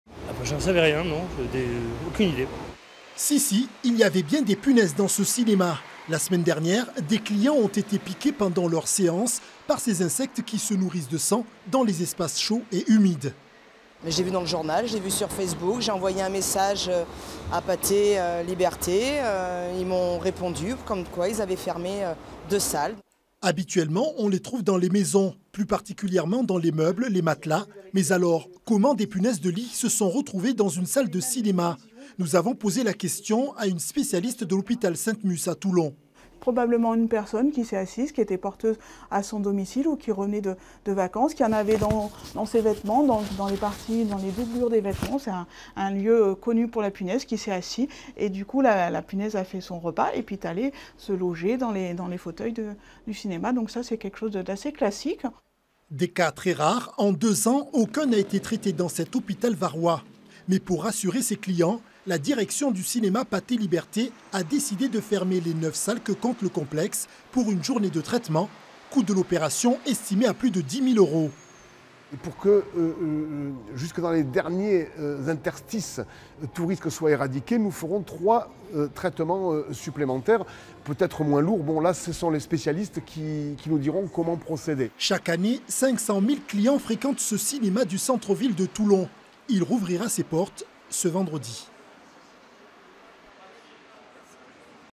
Des punaises de lit dans un cinéma à Toulon
Des clients ont été piqués au cinéma Pathé Liberté à Toulon. Une opération de nettoyage est programmée ce jeudi.
Intervenants : des clients,  Vanessa Van Rossem, chef du service  Hygiène de l'Hôpital de Toulon, Alain Poujol, directeur d'agglomération Pathé Toulon.
Equipe : A.MARTINIKI, M.BAIL, E.MALET
ReportageRetrouvez-nous sur notre site internet :